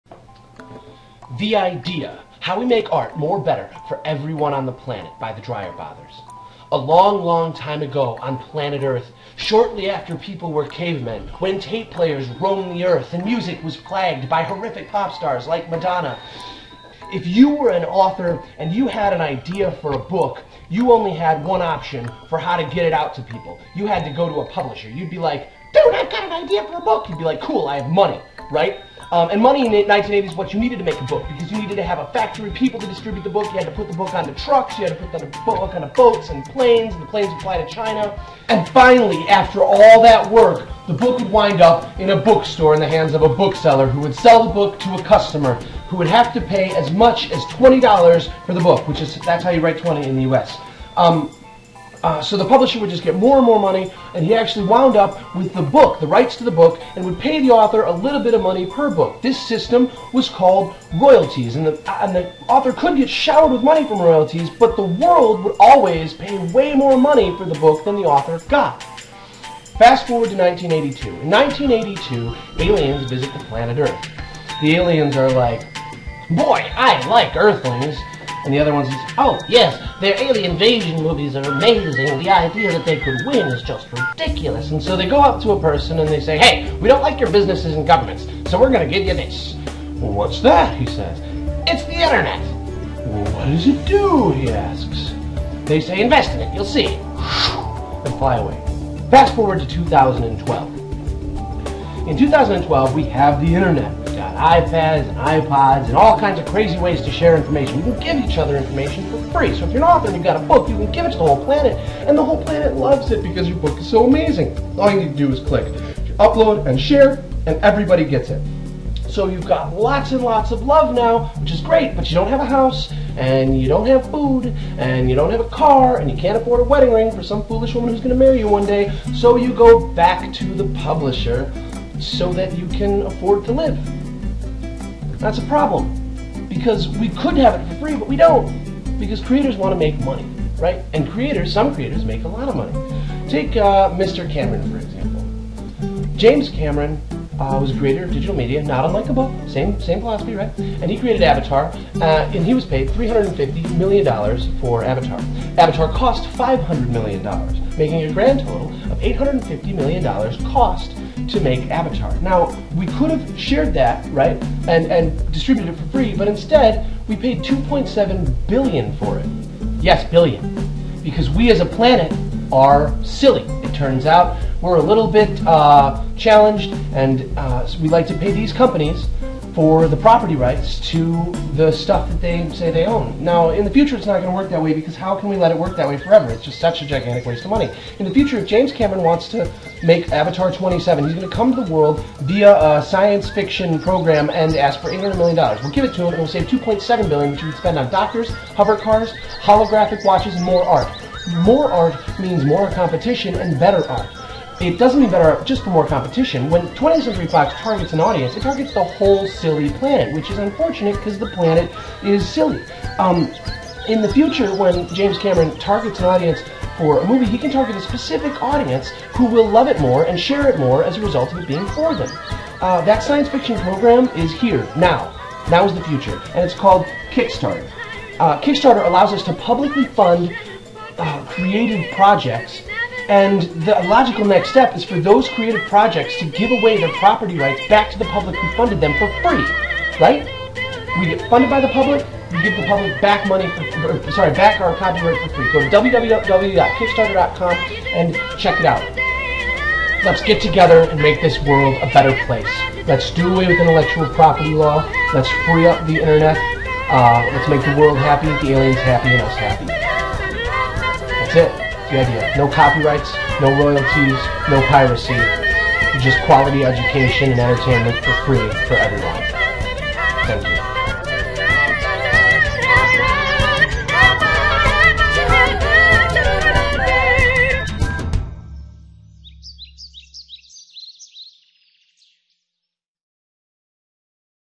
The Idea: How We Make Art More Better for Everyone on the Planet by: The Dreyer Bothers
This is very brief introduction about how to fix piracy, streamline publishing and save the world billions of dollar. We're starting small, very small. Small is how big things start! If you love this Idea, like it, share it and support it.